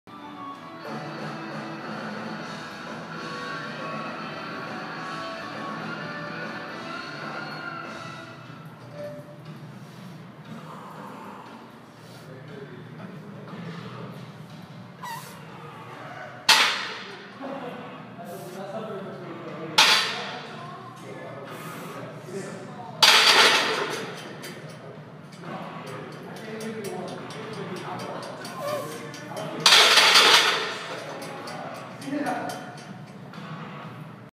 Rack pulls at 405 lbs for 4 reps
5'6" at 177 lbs.  Last working set of rack pulls.